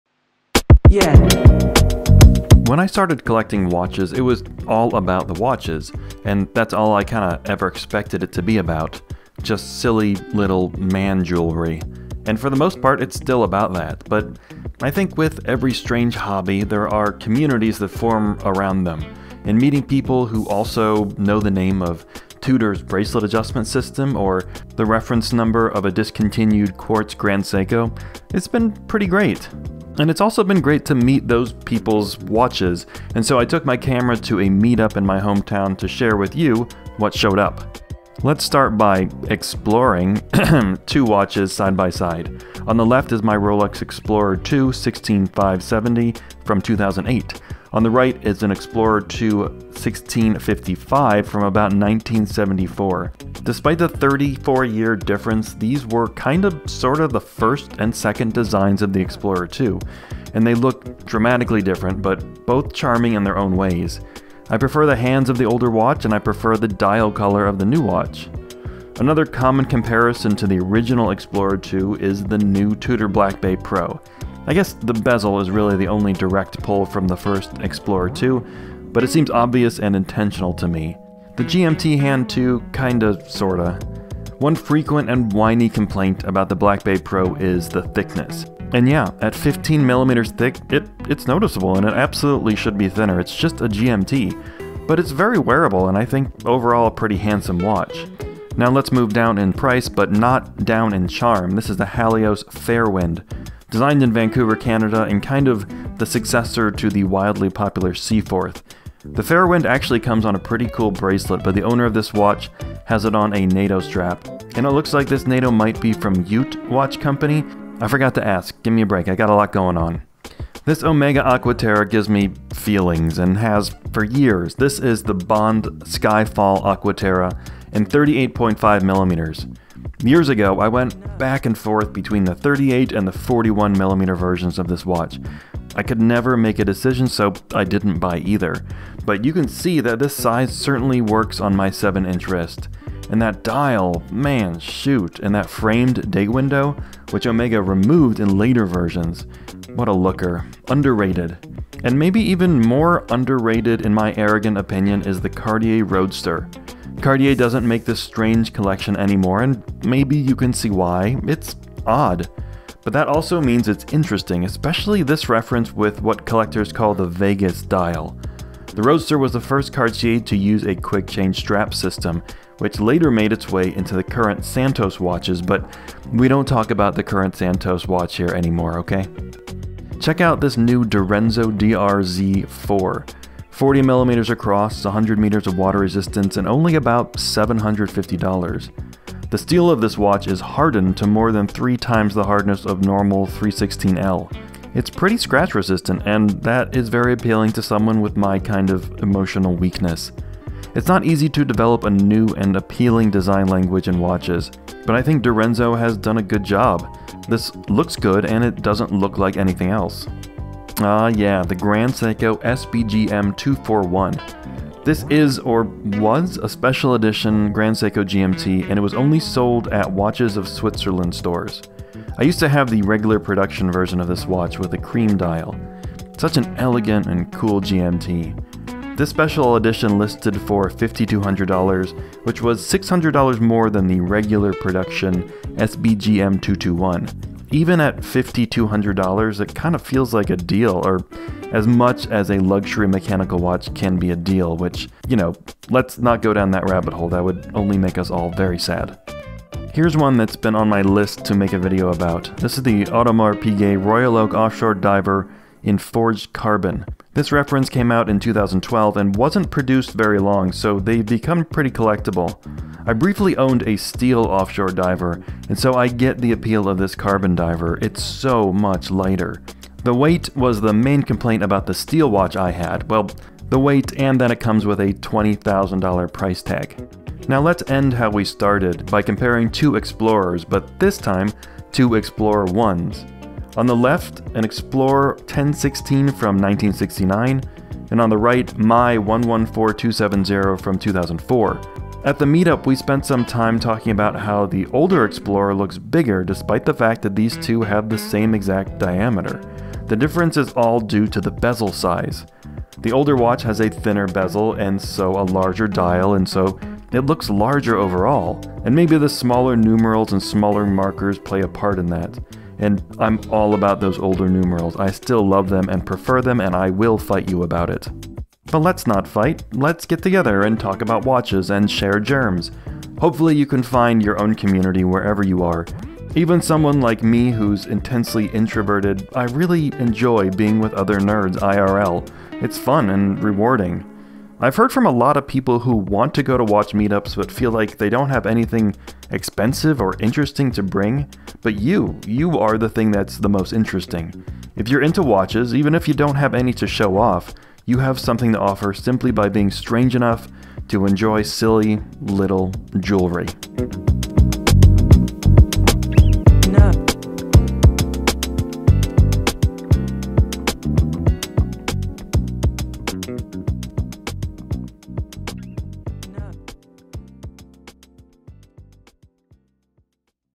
What I found at a Watch Meetup
I went to a local watch meetup. Here's what showed up.

00:00 Intro
00:38 Rolex Explorer II 16570 and 1655
01:08 Tudor Black Bay Pro and Rolex Explorer II 1655
01:38 Halios Fairwind
02:00 Omega Aqua Terra Skyfall
02:32 Cartier Roadster Vegas
03:00 Direnzo DRZ 04
03:30 Grand Seiko GMT SBGM241
04:09 Audemars Piguet Royal Oak Offshore Diver in Forged Carbon
04:38 Rolex Explorer 1016 and 114270
05:18 Outro